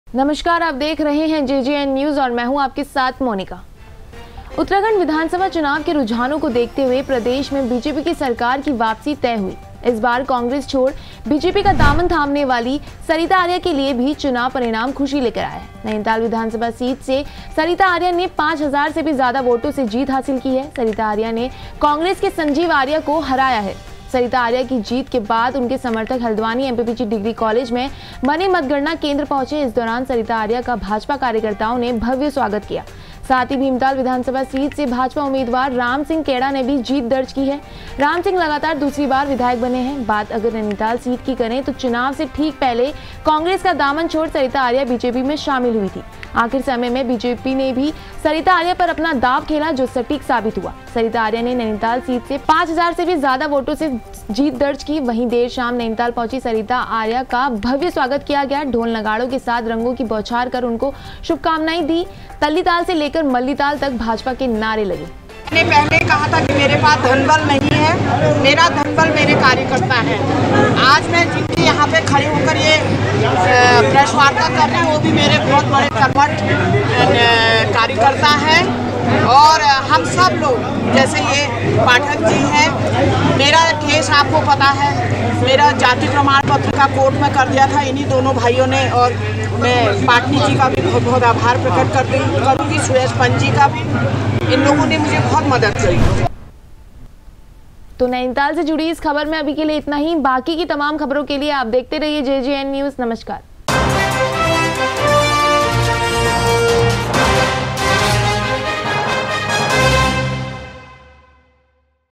नैनीताल जिले की 5 सीटों पर BJP का कब्जा, सरिता आर्य ने मारी बाजी || JJN News
JJN News Nainital,
JJN News,